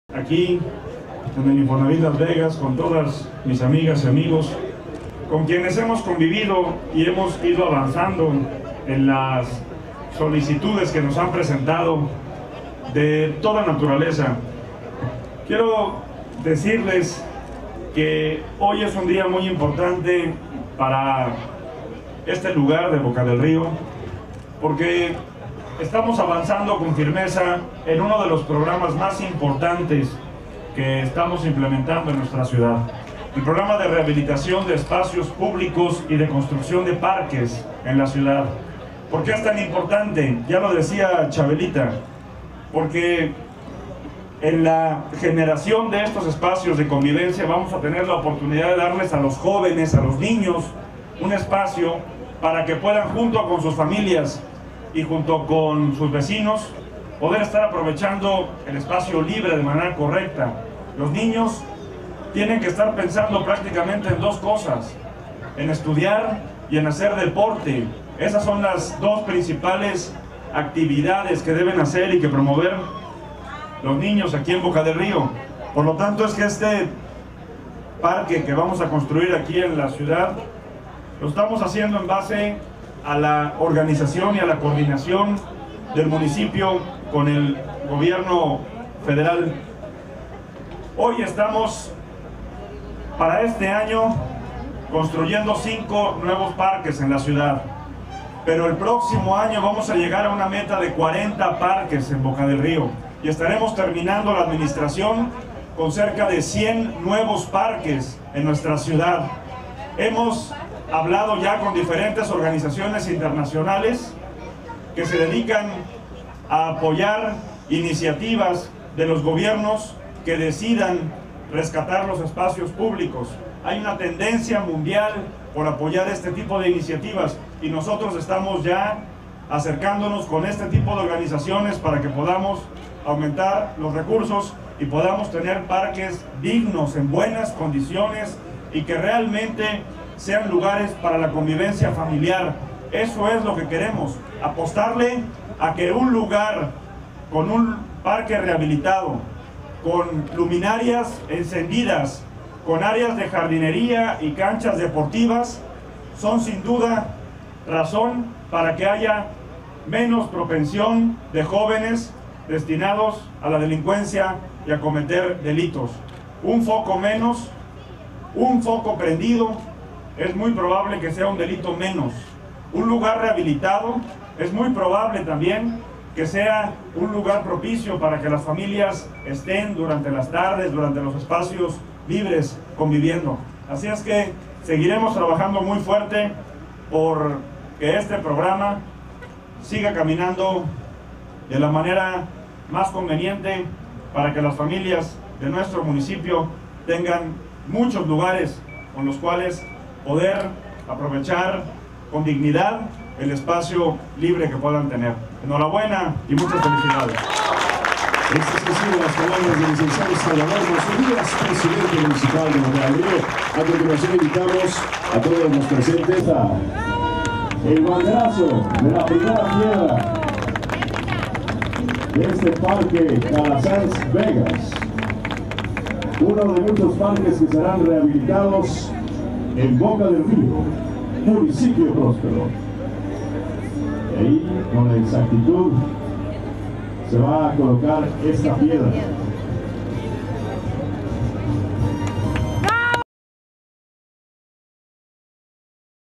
En marcha el Parque de Infonavit Las Vegas- Salvador Manzur
Esta mañana el alcalde Boca del Río, Salvador Manzur Díaz, puso en marcha la rehabilitación del parque en el Infonavit Las Vegas del municipio boqueño.

El parque contará con cancha de futbol, juegos infantiles, nuevas luminarias y andadores, contemplándose concluya los trabajos en un promedio de tres meses.

La inversión será cercana a los dos millones y medio de pesos.

Manzur Díaz, explicó que  su gobierno tiene visualizado la remodelación y construcción de entre 80 y 100 parques en todo su trienio.